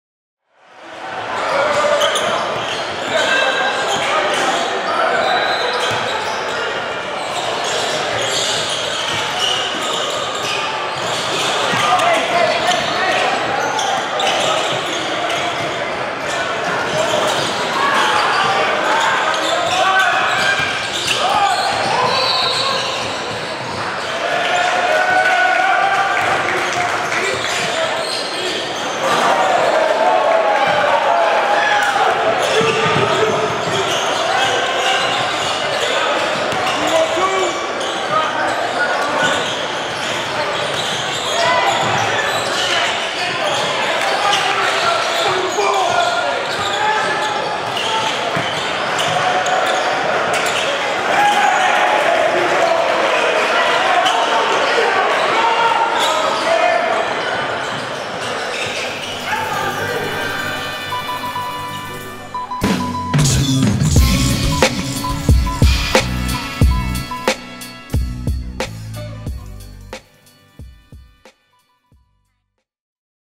Austin Davis vs Micah Potter in 2015 Spiece Run-N-Slam - Michigan & Ohio State commits BATTLE
NextUpRecruits